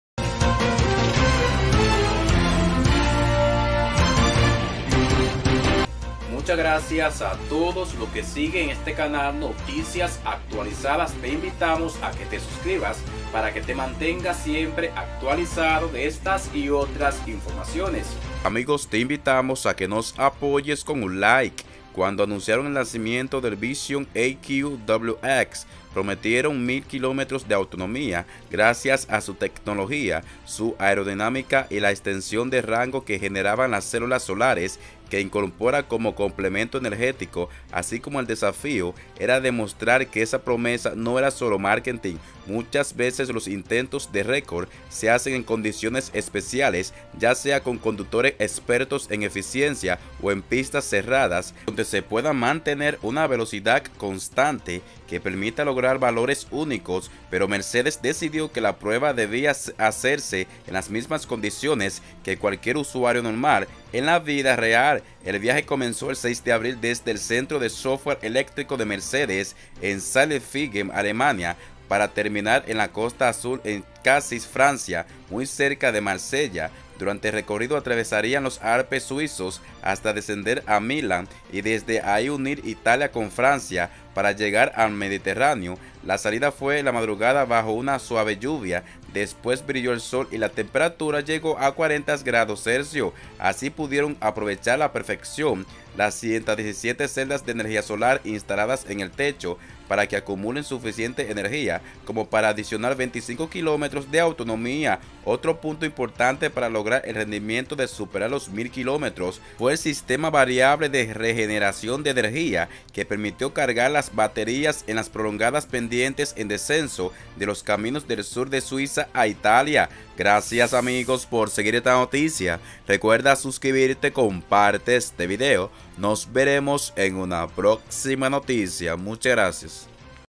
EL AUTO ELÉCTRICO RÉCORD QUE SUPERÓ LOS 1.000 KM SIN DETENERSE PARA CARGAR LA BATERÍA
Del otro lado, los fabricantes tradicionales, las grandes marcas de toda la vida, siguen dando señales contundentes como respuesta a los usuarios. Ford acaba de confirmar que el próximo 26 de abril empieza la fabricación de la F-150 Lightning, la pickup 100% eléctrica que se ha transformado en el producto más esperado de la marca en décadas, y que ya tiene vendida la producción de los primeros tres años.

Paralelamente, en Alemania, Mercedes-Benz decidió dar un paso gigante como respuesta al crecimiento de Tesla, y lejos de asustarse, pusieron a prueba su novedoso concept car VISION EQXX, que habían presentado apenas en enero. Generalmente, del auto de concepto al auto real suele pasar bastante tiempo, durante el cual también cambian muchas cosas de su diseño original.

En este caso, el mismo prototipo construido a mano, fue el utilizado para la prueba de autonomía.

Cuando anunciaron el nacimiento del VISION EQXX, prometieron 1.000 kilómetros de autonomía gracias a su tecnología, su aerodinámica y a la extensión de rango que generaban las células solares que incorpora como complemento energético. Así que el desafío era demostrar que esa promesa no era solo marketing.

Muchas veces los intentos de récord se hacen en condiciones especiales, ya sea con conductores expertos en eficiencia, o en pistas cerradas donde se puede mantener una velocidad constante que permita lograr valores únicos. Pero Mercedes decidió que la prueba debía ser hacerse en las mismas condiciones que cualquier usuario podría encontrar en la vida real, con el tránsito variable, con clima adverso si se presentaba y con conductores normales. Nada excepcional.